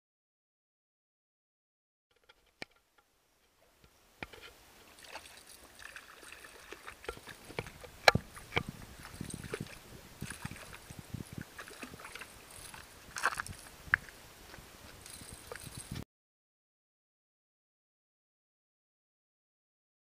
jack Cravelle two on a chug bug LOL
Early  morning Jack bite in the back yard canal LOL